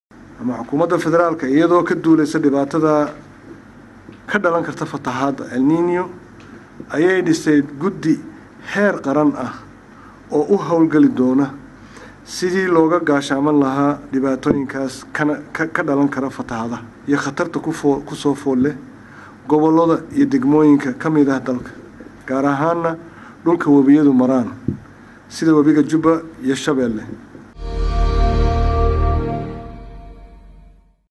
RW Ku Xig. Somalia Maxamed C. Arte oo ka hadlay Guddiga loo saaray Gurmadka Fatahaada
Golaha Wasiirada ayaa ugu dambeyntii isla qaatay in la dhiso guddi heer Wasiir ah, kaasoo howshiisu noqon doonto xiriirinta dowladda Federaalka, maamul goboleedyada iyo hay’addaha caalamiga ah, waxaana Ra’iisul Wasaare kuxigeenka Mudane Maxamed Cumar uu xusay in arrintani ay tahay musiibo ku soo wajahan dalka, loona baahan yahay inay xil iska saaraan dhamaamba qeybaha kala duwan ee dadka Soomaaliyeed, gudaha iyo dibedda, ayna gacan xukuumada gacan ku siiyaan sidii loo yareyn lahaa dhibaatooyinka ka dhalan kara.